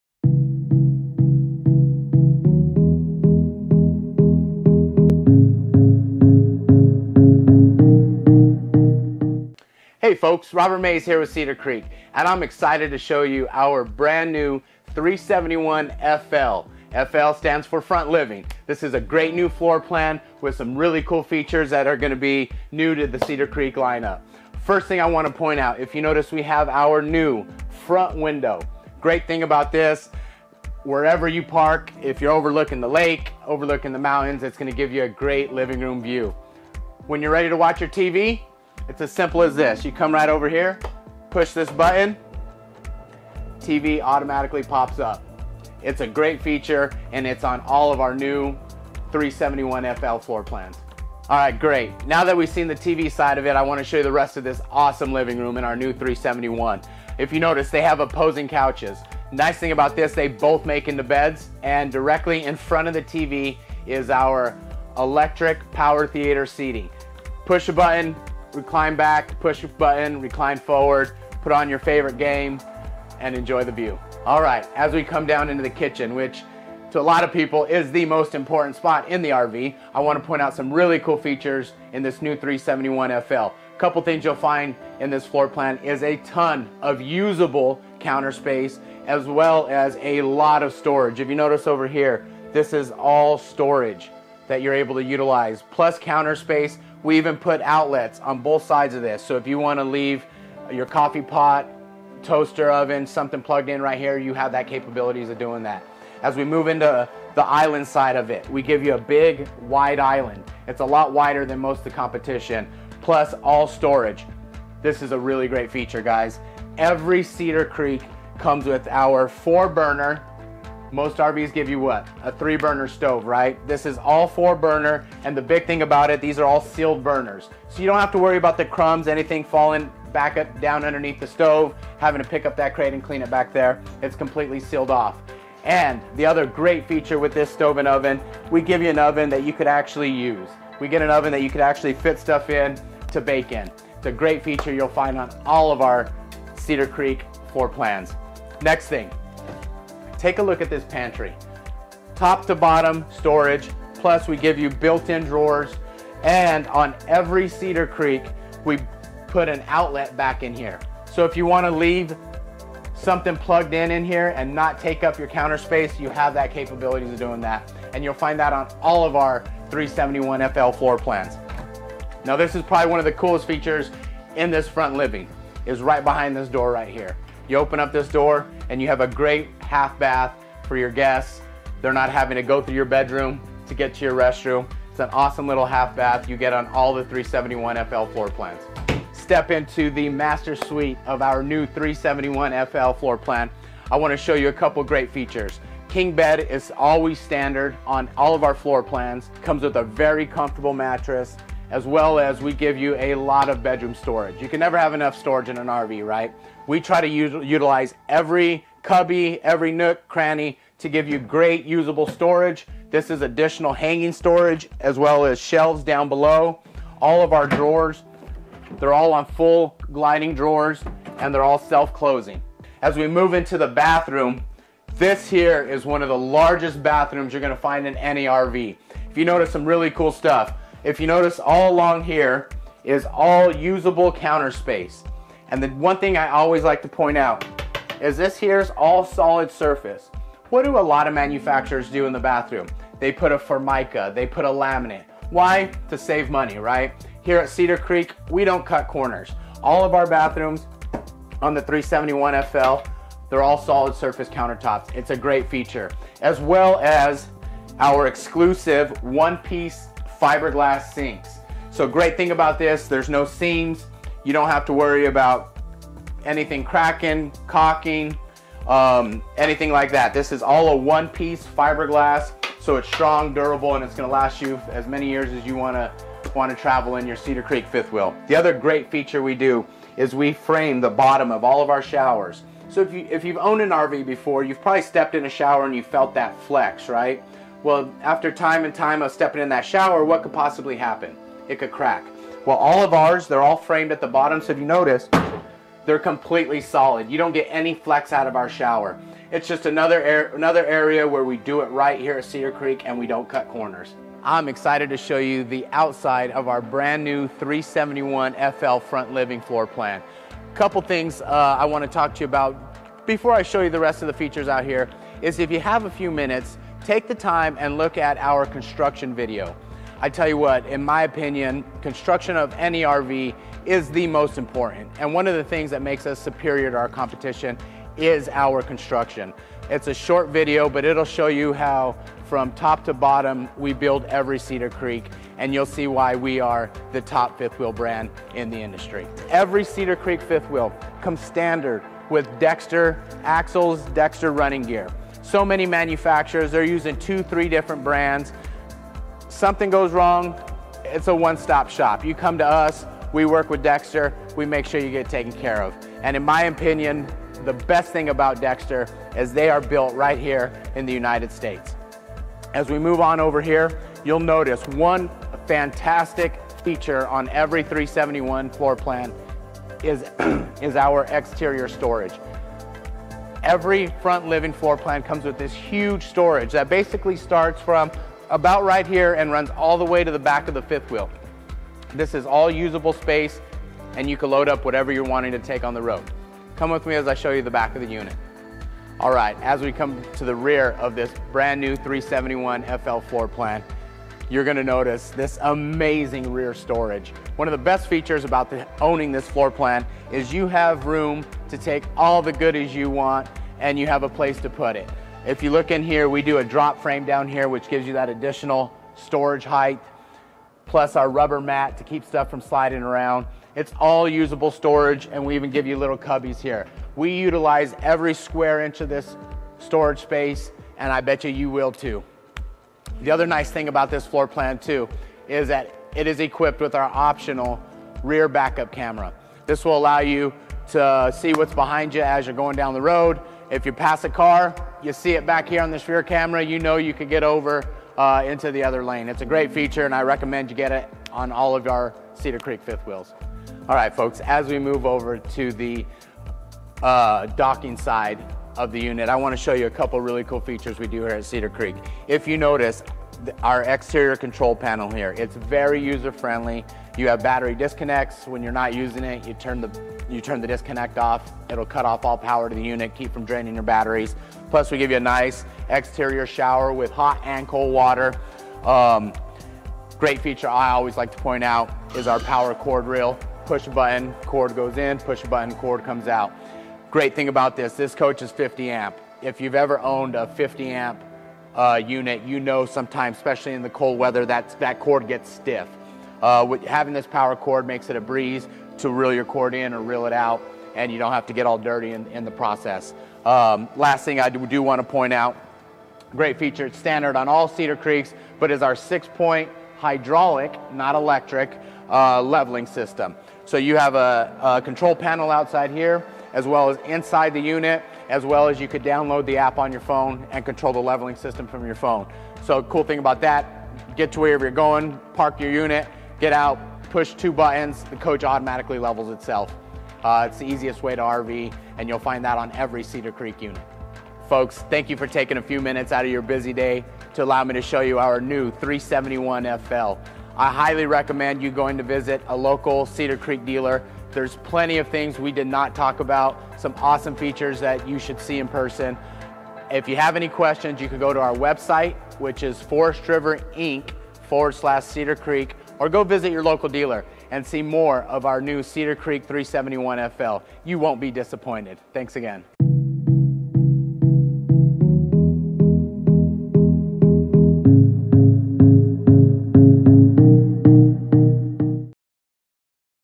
2021 Cedar Creek 371FL Walkthrough
The Cedar Creeks 371FL is the perfect fifth-wheel for entertaining. The front living room can seat up to 6 people!